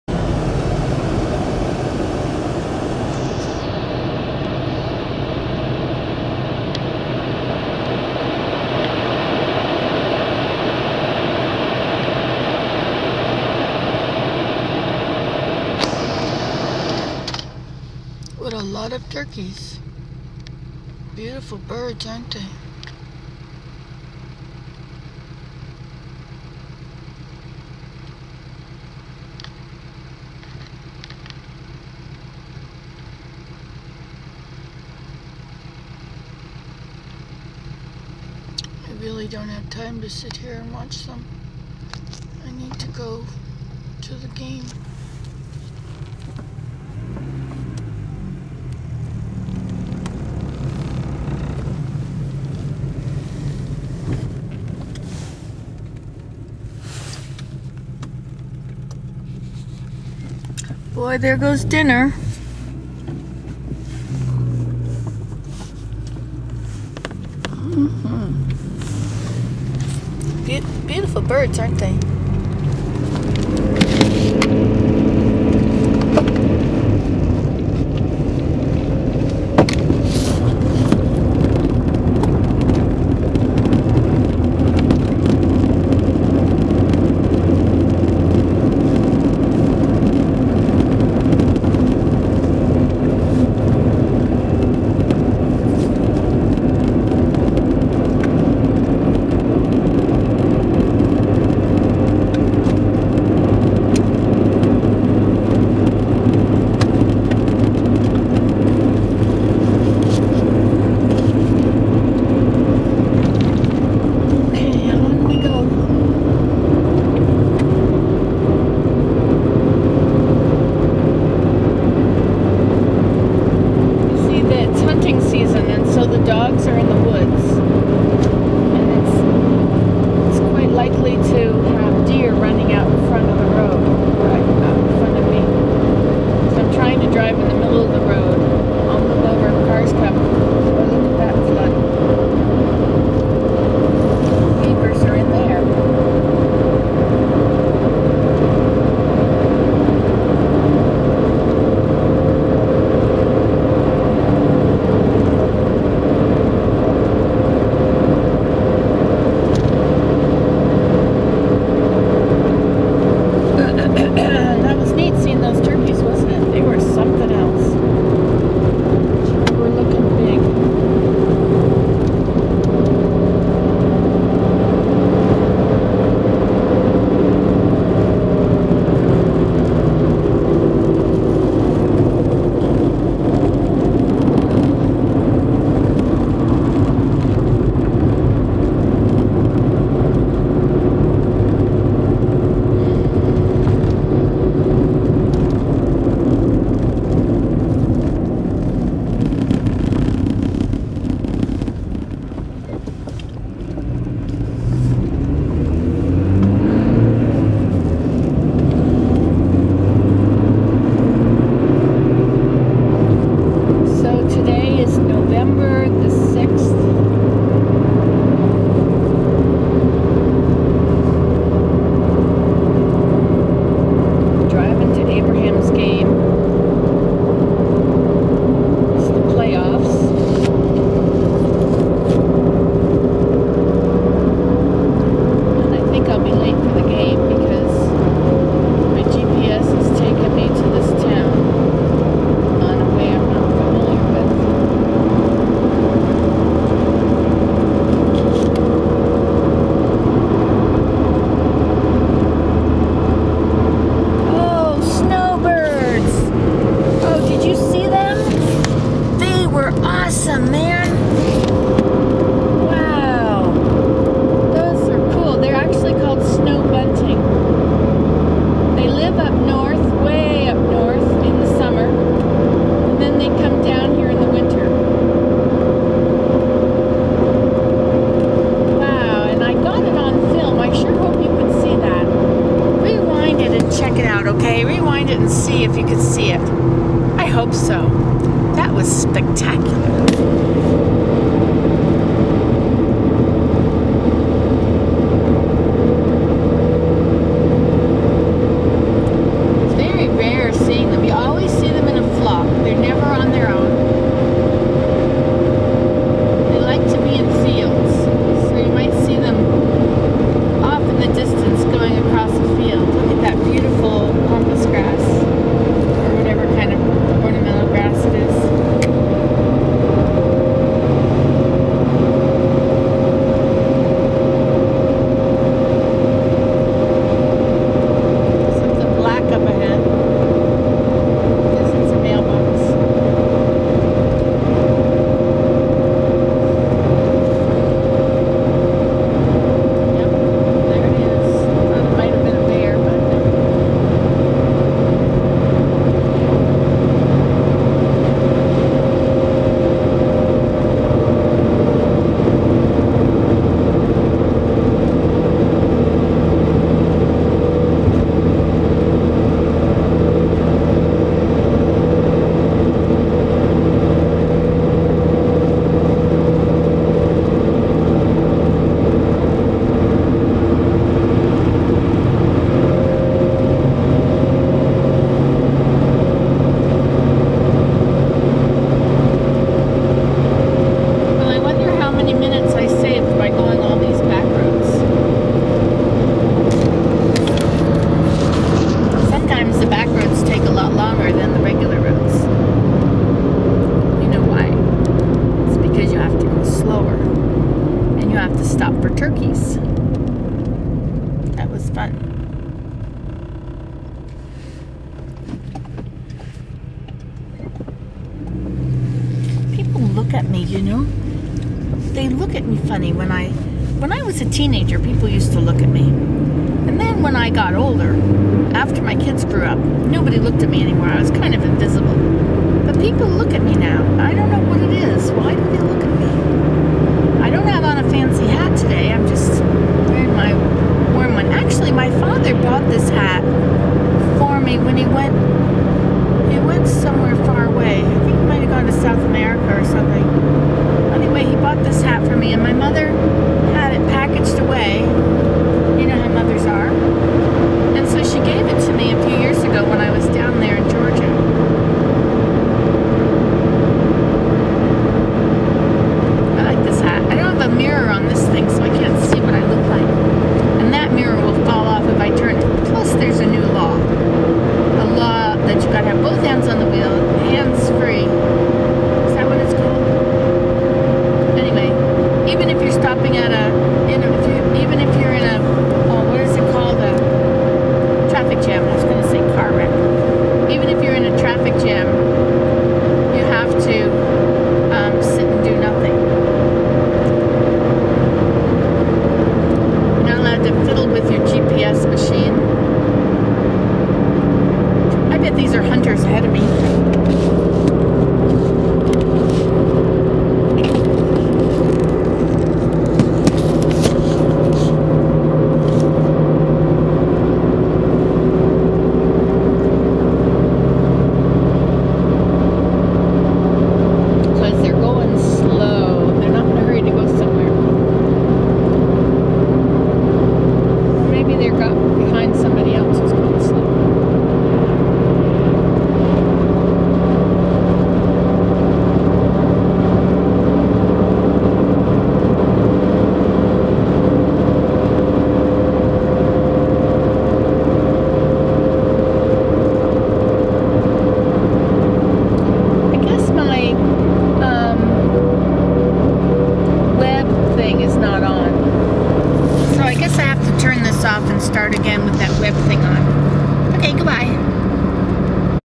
Nancy Today: Turkeys and Snow bunting at 4:11 ASMR Birds 'Sounds of Nature'
I love snow birds.  they are just lovely little birds.